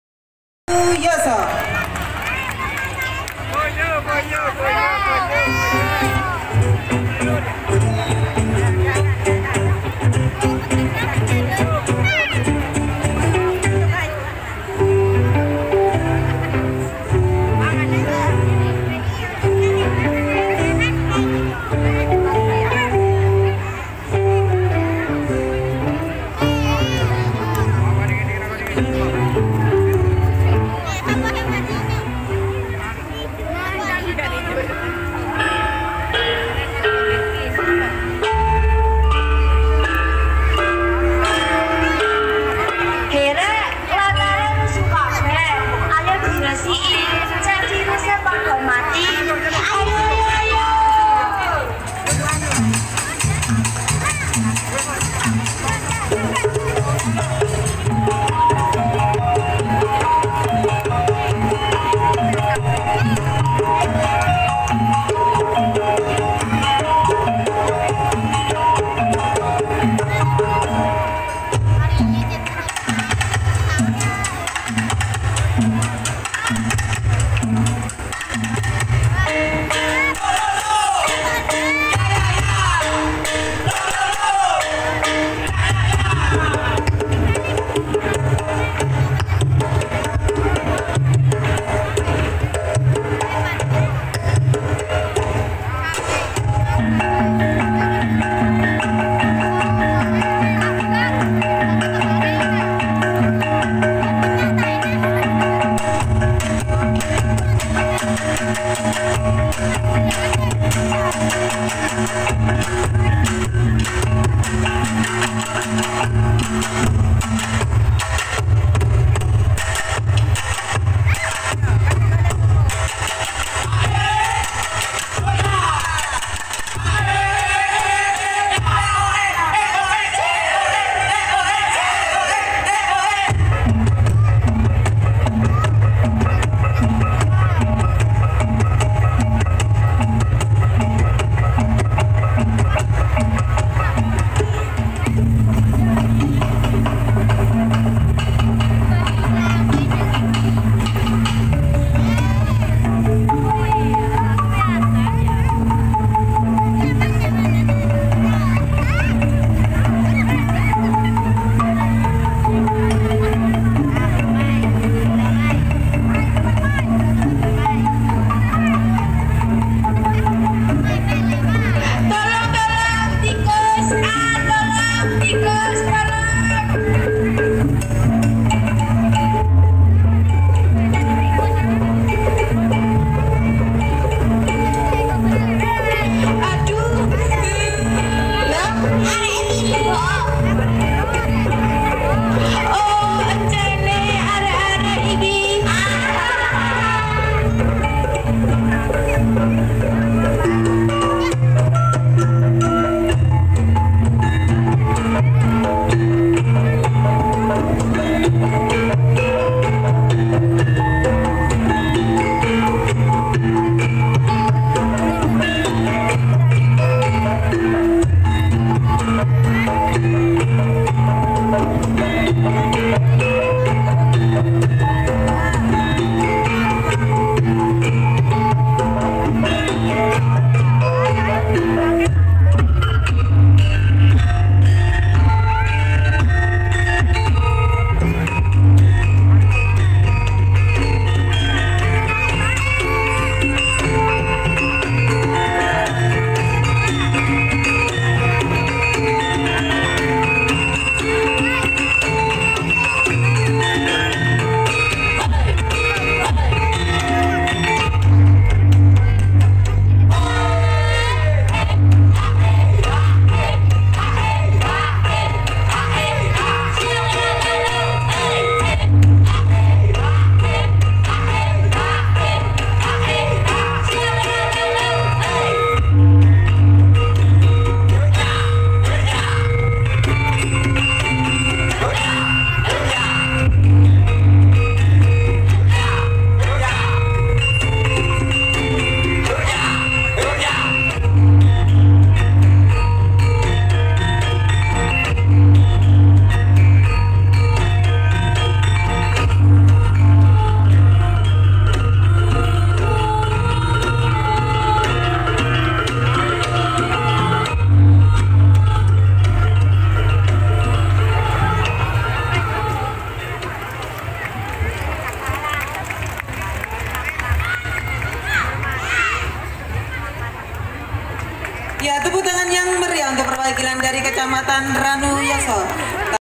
Tari Tradisional kolaborasi drama( Kecamatan Ranuyoso) Gebyar Pol Polan Lumajang
Tari tradisional kolaborasi drama oleh kecamatan Ranuyoso